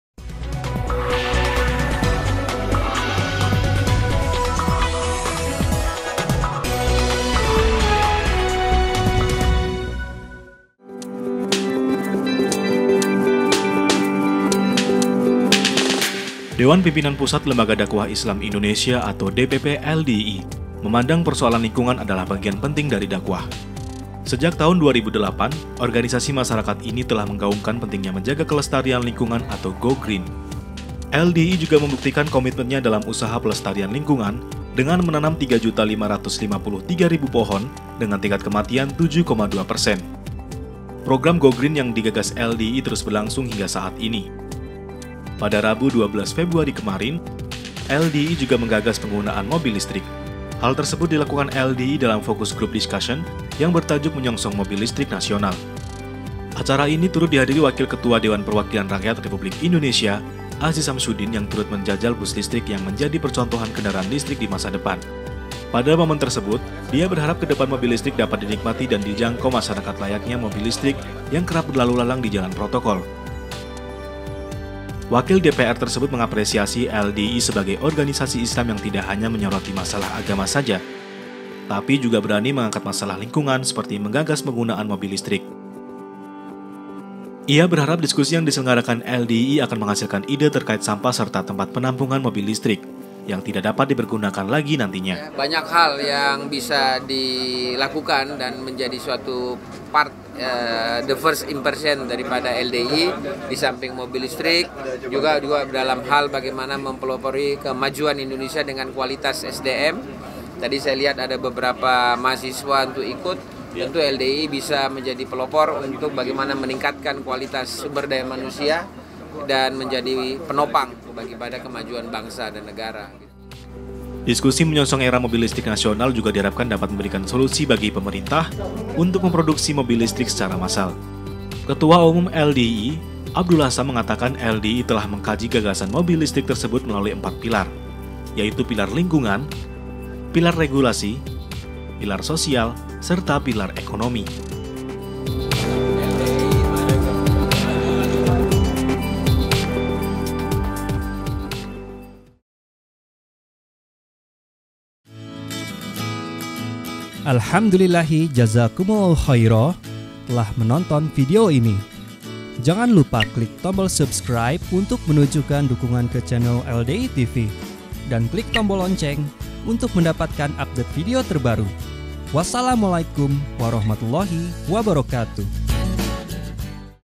LDII TV : LDII Sambut Era Mobil Listrik Nasional
LDII kembali menggelar Focus Group Discussion mengenai pembuatan mobil listrik sebagai solusi masyarakat, yang bertempat di Kantor DPP LDII, Patal Senayan.

Ketua Umum DPP LDII, Prof. Dr. Ir. KH. Abdullah Syam M.SE. dalam sambutannya mengatakan, ia menyoroti pembuatan mobil listrik dilihat dari tiga pilar; pertama, apakah layak pada lingkungan, layak teknisnya, layak terhadap sosial atau tidak.

Seperti yang diketahui, adanya pemanasan global serta perubahan iklim yang diakibatkan ekosistem hutan semakin berkurang, keragaman hayati semakin berkurang, yang pemanfaatannya kurang bijak. Ekosistem yang tidak seimbang.

Dan di sisi lain industri mobil hingga kini masih menggunakan bahan bakar yang berasal dari fosil, yaitu batu bara. Yang akan meningkatkan akumulasi karbon monoksida, sehingga merusak lapisan ozon sebagai filter sinar ultraviolet ke bumi.

Karena itu, jika pelaku industri fokus kepada mobil listrik, setidaknya sumber daya manusia juga bisa mengurangi masalah karbon monoksida tersebut. Terkait masalah lingkungan, mobil listrik dianggap sebagai salah satu solusi mengurangi dampak negatif dari penggunaan bahan bakar fosil.Lebih lengkapnya bisa di cek di website